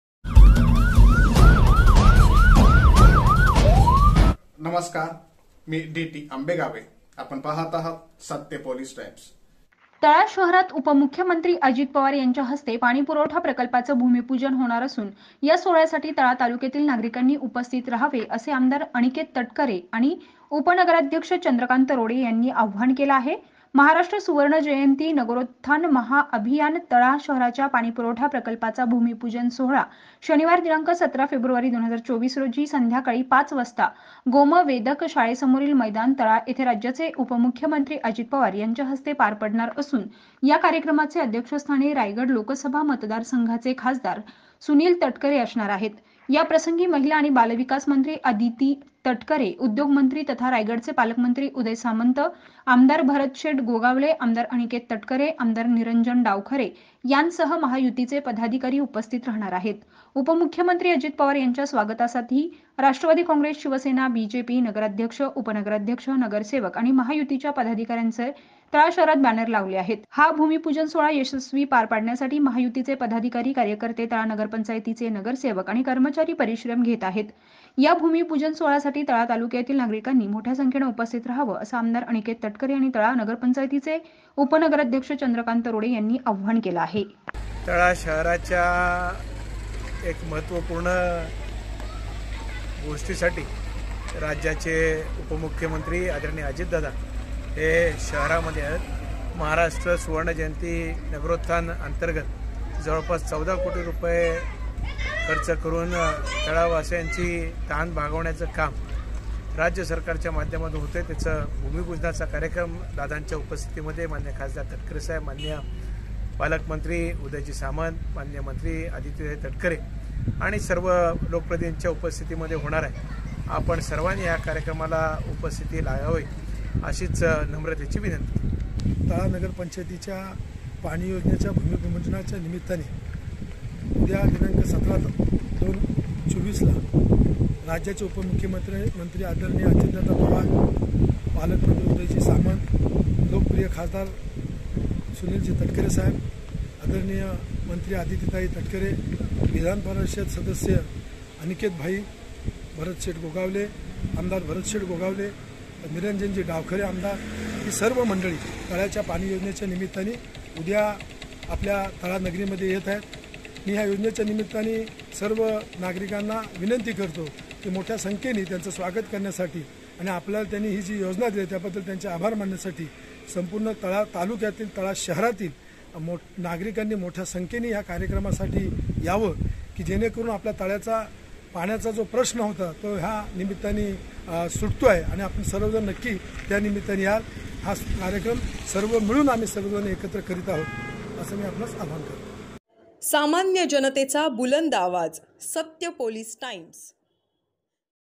Tala पाणीपुरवठा प्रकल्पाच्या भूमिपूजन सोहळ्यासाठी MLA Aniket Tatkare व चंद्रकांत रोडे यांचं आवाहन!
Tala शहरातील पाणीपुरवठा प्रकल्पाच्या भूमिपूजन सोहळ्यासाठी MLA Aniket Tatkare व चंद्रकांत रोडे यांचं जनतेला आवाहन!

सामान्य जनतेचा आवाज बुलंद करण्यासाठी